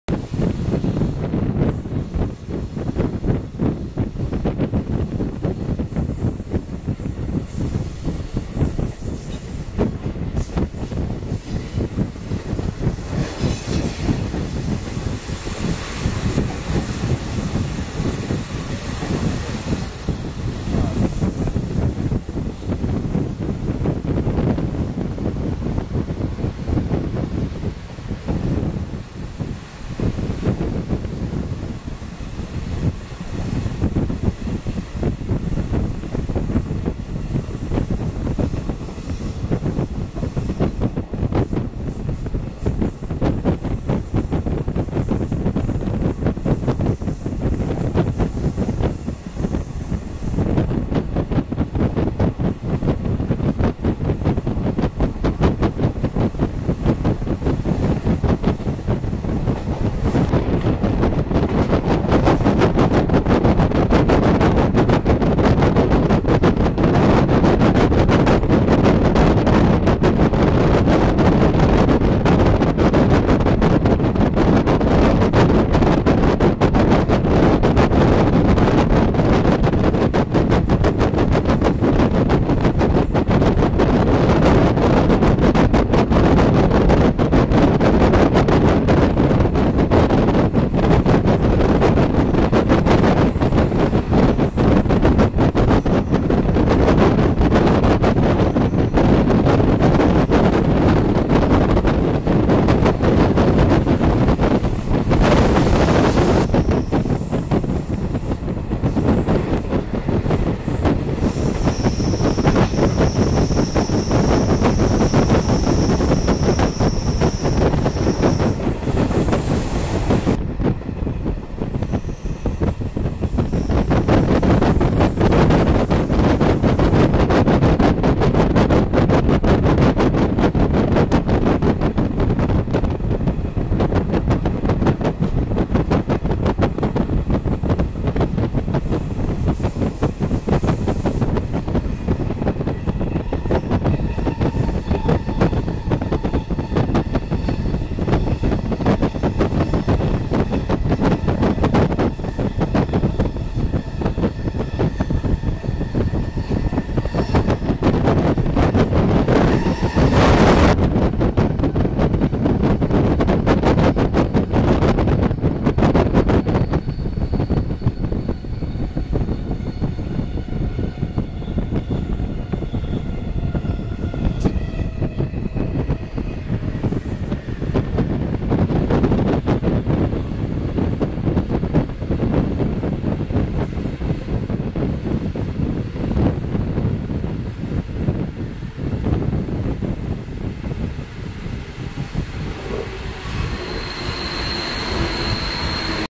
Sveriges vackraste järnväg ligger i Norge
Utsikt från tågfönstret på Malmbanans norska del mellan Katterat och Søsterbekk.